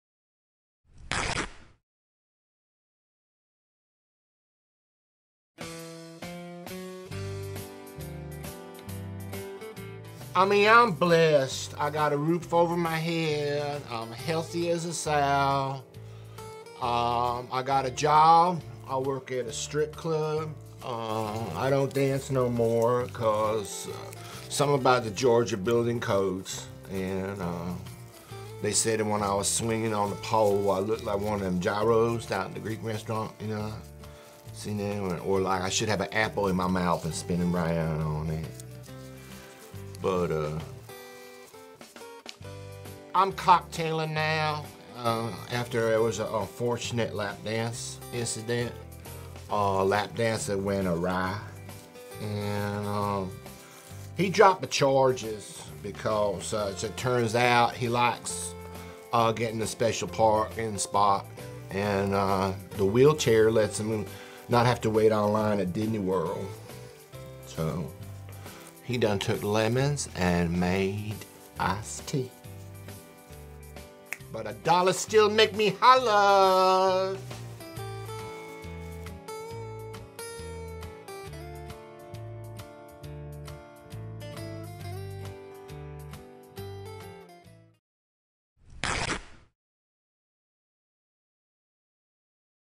Boo Hoo for Honey Boo Boo Episode 5
Your Go-2 Destination for sketch comedy

Alana brings us up to date with what she’s been doing to make a living since leaving the Pageant World.

Actor:
Rob Bartlett